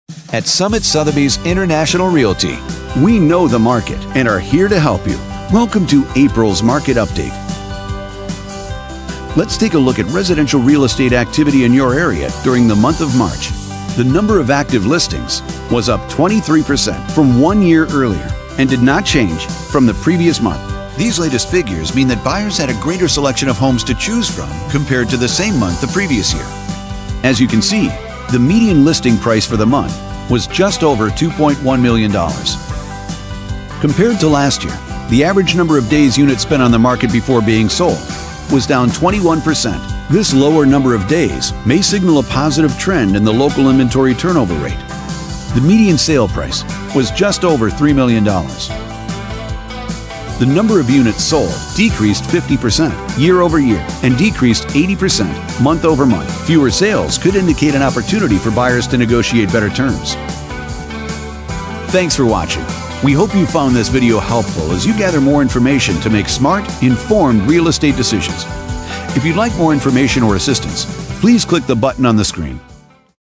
Upper Deer Valley Resort - Condo - April 2019 Real Estate Market Update
Real estate is local, which is why we offer you current and historical data from our local Park City MLS to produce detailed, highly visual and easy to interpret videos that clearly demonstrate market trends for Upper Deer Valley Resort - Condo.